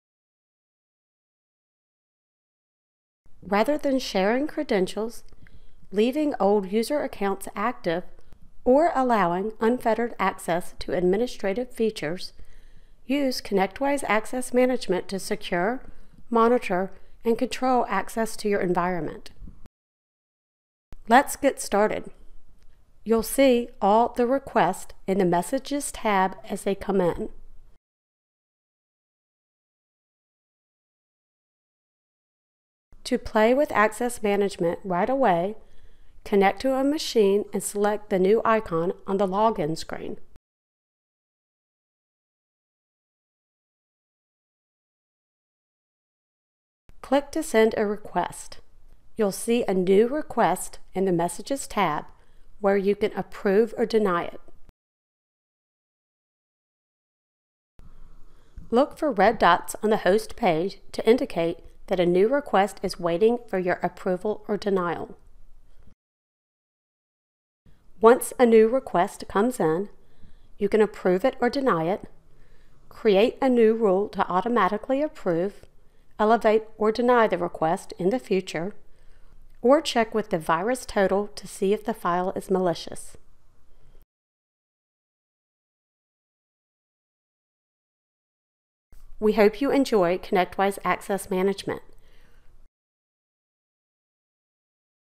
ConnectWise Access Management Quick Start Video
Get started with ConnectWise Access Management with this video and our quick start guide